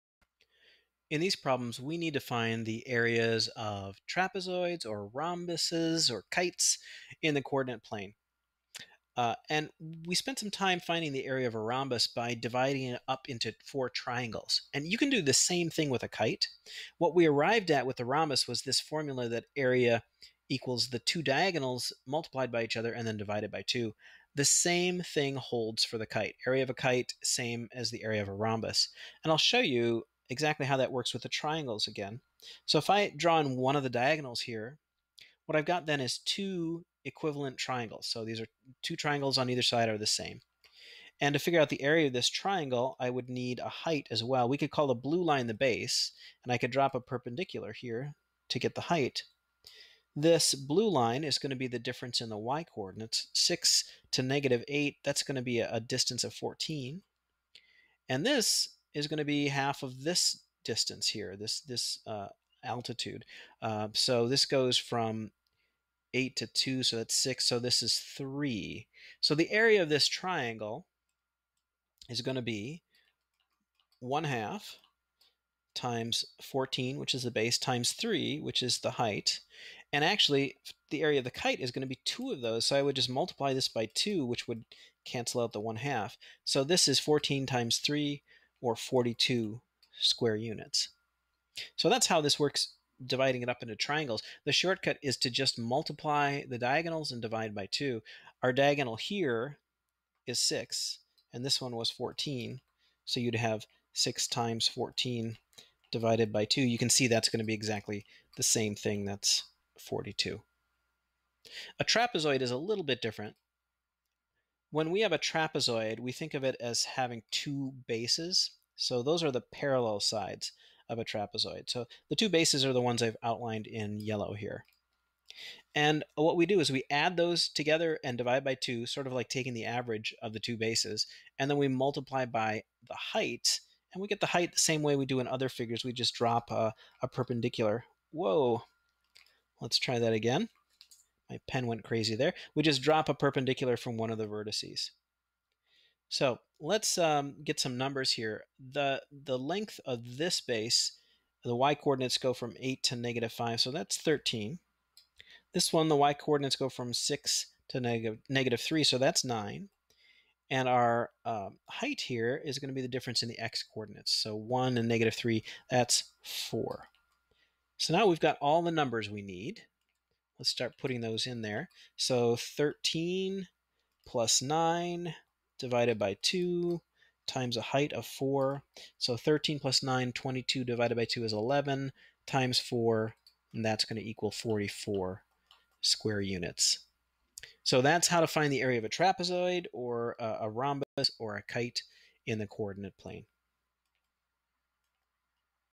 Finding the area of a trapezoid, rhombus, or kite in the coordinate plane
How to find the area of a trapezoid, rhombus, or kite in the coordinate plane